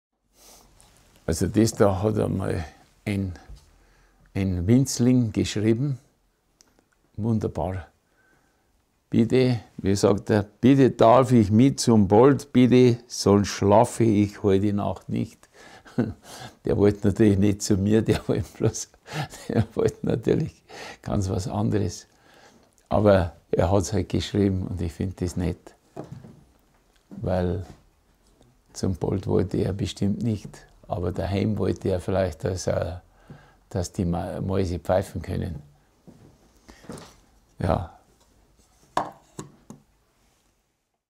Der Winzling
Gerhard Polt erklärt eines der Exponate der Ausstellung »Braucht"s des?! Gerhard Polt zum 70sten« Eine Ausstellung des Literaturhauses München für und mit Gerhard Polt (2.3. bis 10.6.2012)

Ein Film von Sandra Wiest, Kuratorin der Ausstellung